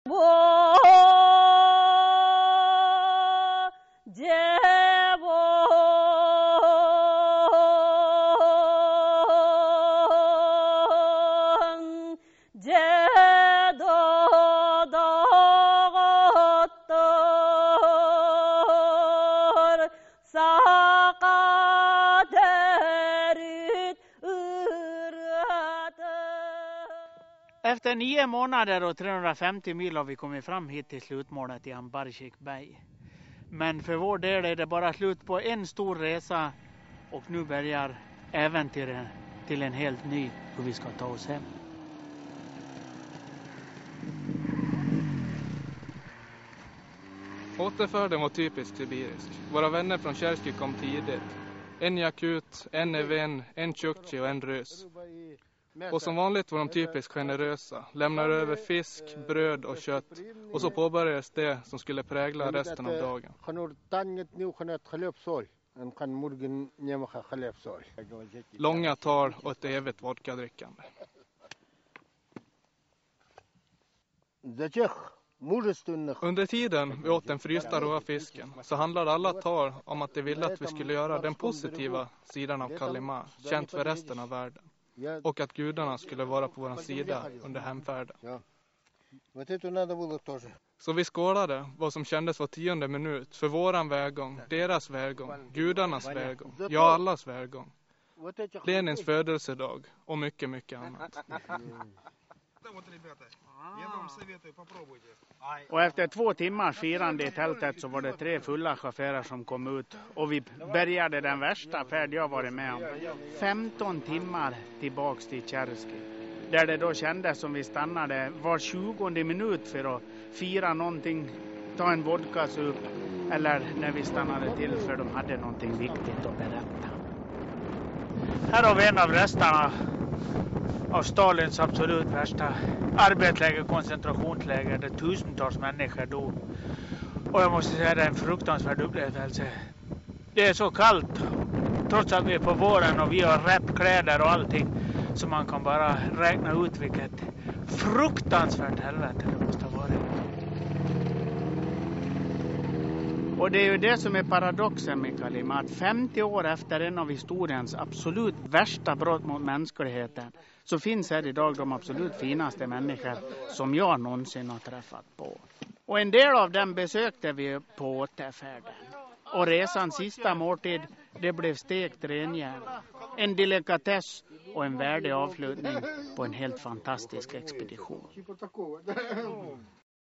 The last stretch on the Kolyma Expedition
Explorers Johan Ivarsson and Mikael Strandberg reach Ambartchik Bay after 10 months of travelling along the Kolyma River, northeastern Siberia